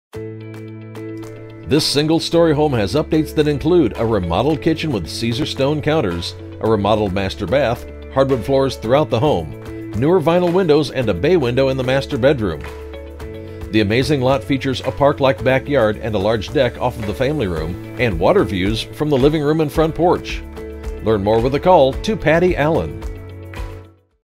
1456 Charmian Court Benicia, CA
1456 Charmian Court, Benicia, CA

This single-story home has been tastefully updated throughout its lifetime. The updates include a beautiful remodeled kitchen with Caesar-stone counters and stainless appliances, a remodeled master bath, hardwood floors throughout the home, newer vinyl windows including sliding glass doors and a bay window in the master bedroom. The amazing approximately one-third acre lot features a park-like backyard, a grape arbor and peaceful sitting areas. There is a large deck off of the family room and water views from the living room and front porch.